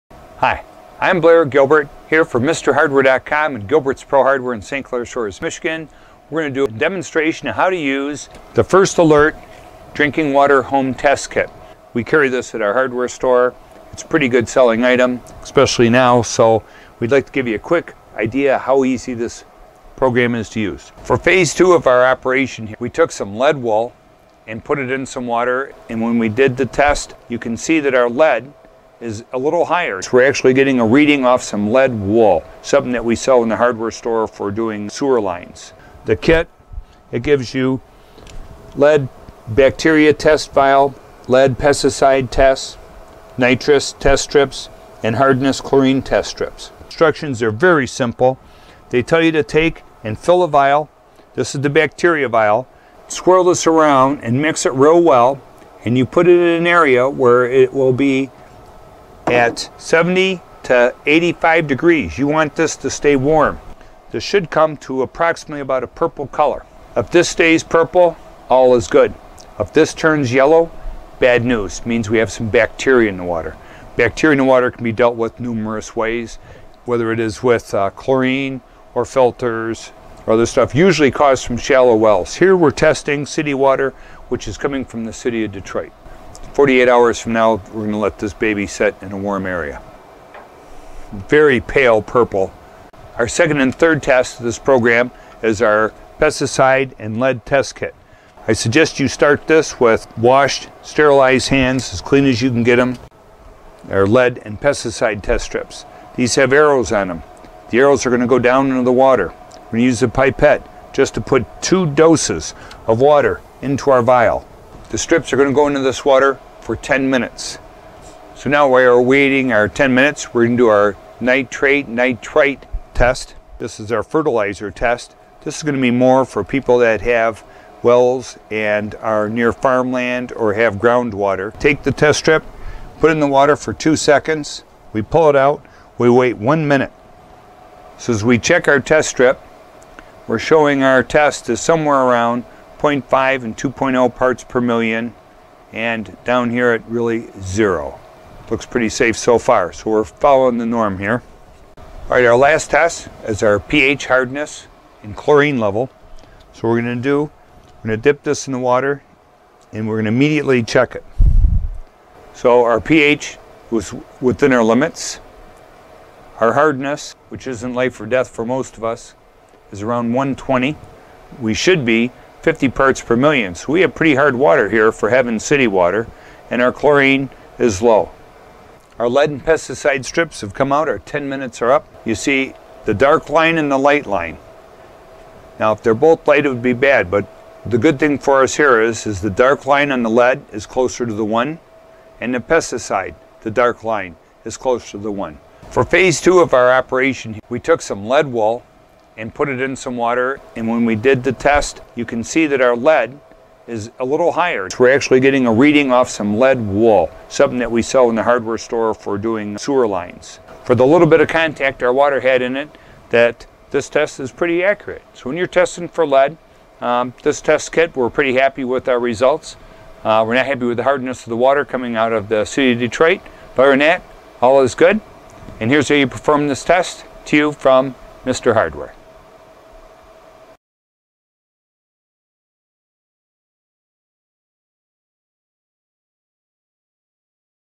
Is Your Home’s Water Safe? Lead Water Test Kit Results by First Alert
How to use 4-test water kit, and adding lead to prove a positive result. We have Detroit city water and expected excellent scores, but the test showed we have hard water. What's up with that?